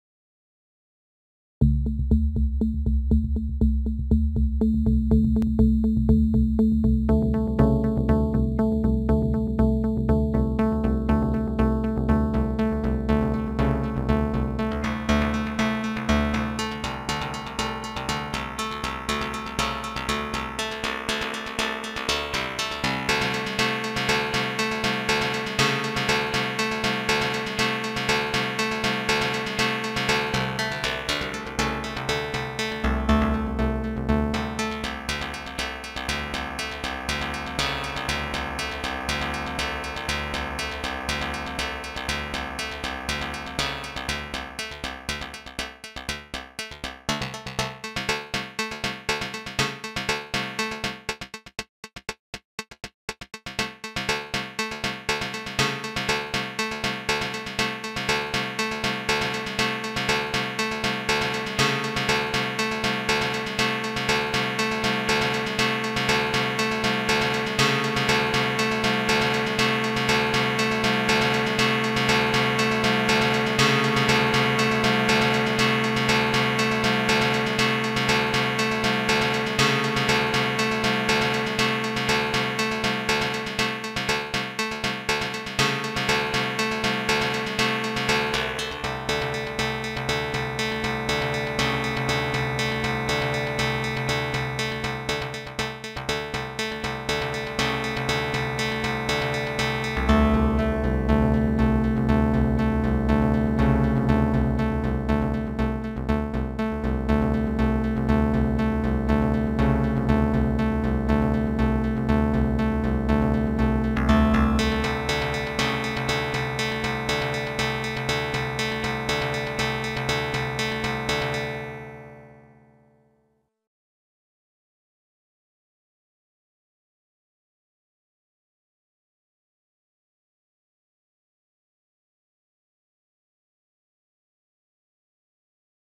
FREE Karp by Nathan Blair
It is a plucked string synthesizer based on the classic Karplus-Strong algorithm.